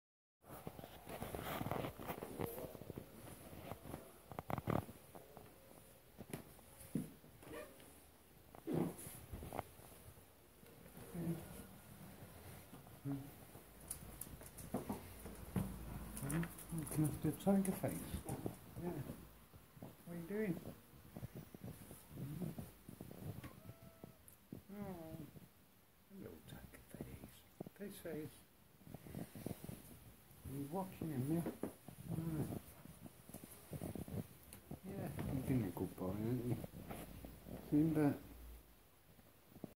Blue roan cocker spaniel protects cat
He guards the stairs so his other doggy friends don't disturb him